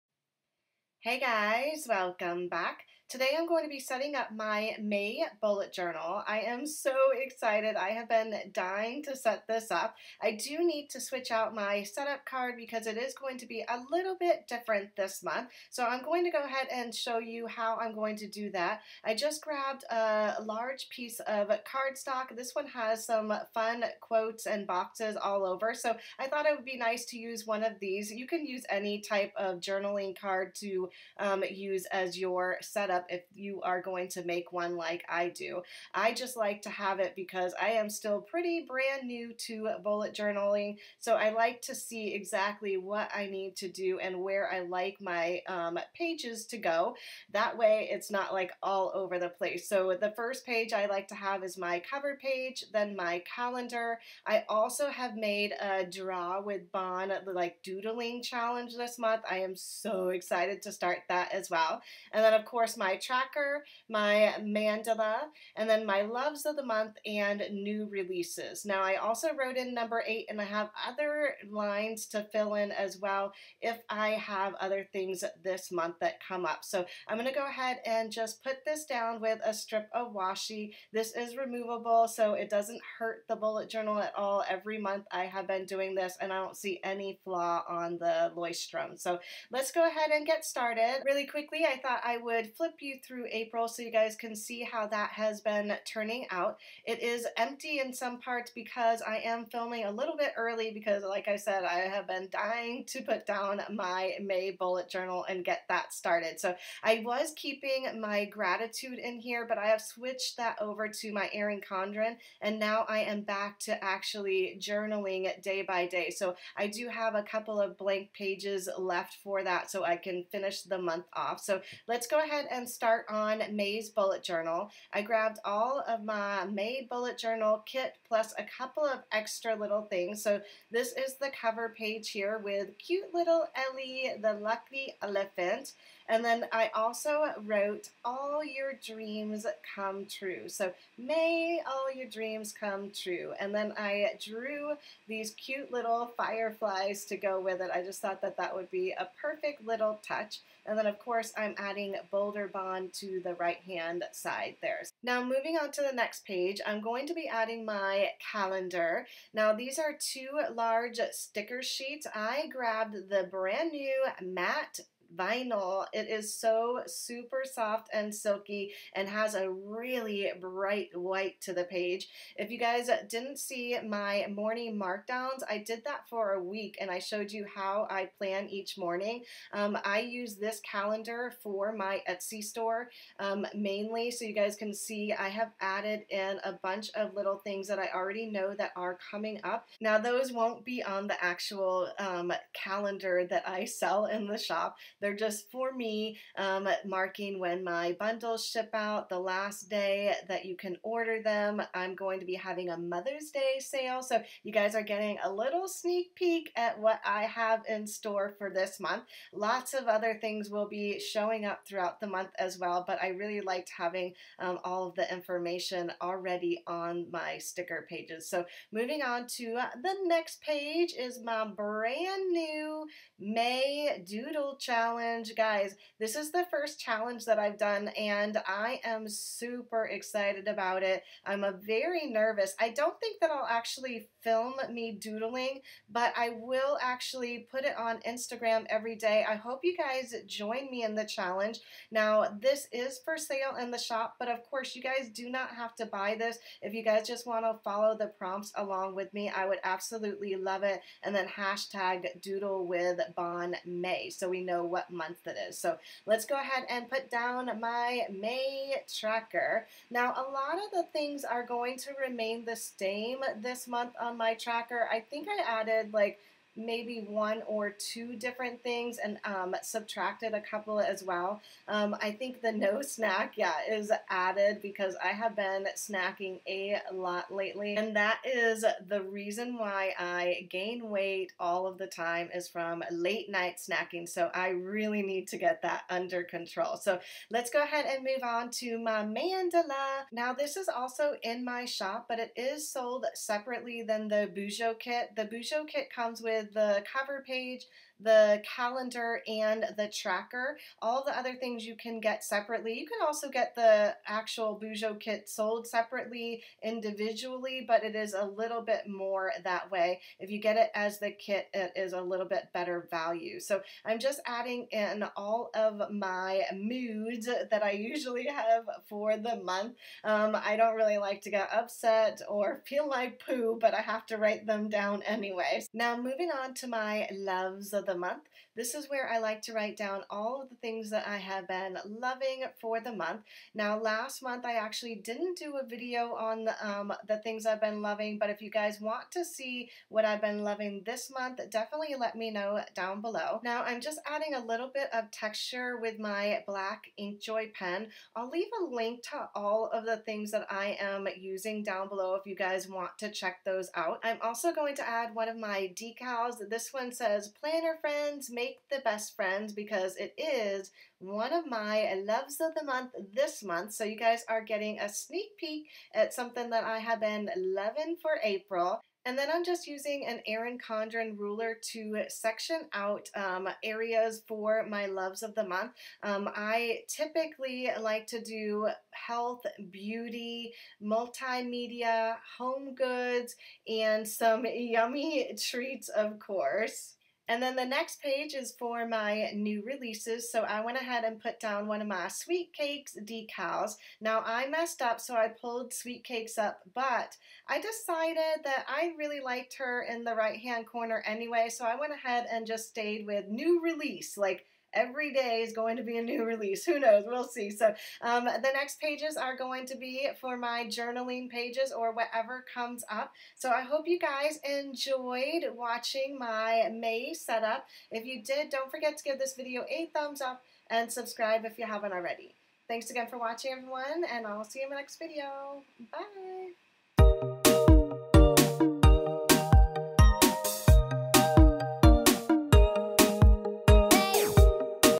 Bullet Journal May Setup | Plan With Me
Loving this month's bullet journal layout. Today I'm sharing my May bullet journal setup. In this Plan With Me I use a mix of stickers, drawing, and doodling.

Join me for May's doodle challenge! Make sure to tag me on Instagram, and doodlewithbonmay or join my Facebook group to share pictures.

Micron Pens ●

Paper mate InkJoy pens

Crayola Markers

Sharpie Metallic Markers

● Equipment Used ●
Camera: Canon Rebel T5i ●